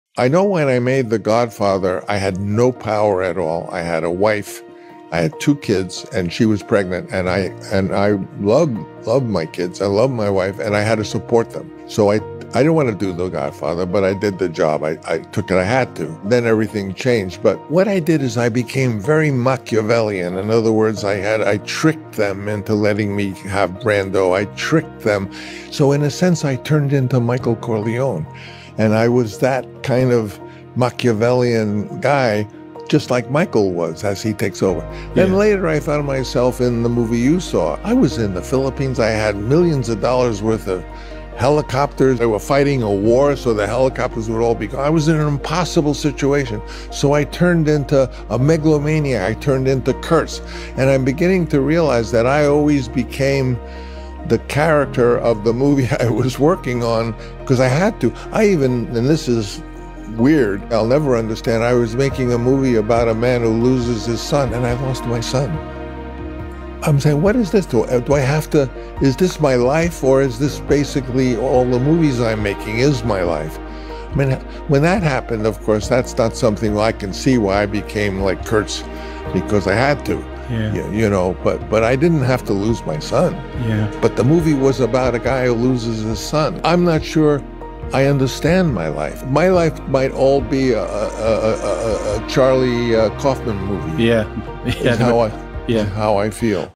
FRANCIS FORD COPPOLA'S SHOCKING CREATION SECRETS REVEALED!
Get ready to uncover the secrets behind the mastermind of Francis Ford Coppola, the legendary director of The Godfather and Apocalypse Now! In this video, we'll delve into the shocking creation secrets that made his films iconic and unforgettable. From his unorthodox writing techniques to his innovative filmmaking approaches, we'll explore the methods that set him apart from other directors. Whether you're a film enthusiast, a budding filmmaker, or simply a fan of Coppola's work, this video is a must-watch. So, sit back, relax, and get ready to have your mind blown by the shocking creation secrets of Francis Ford Coppola!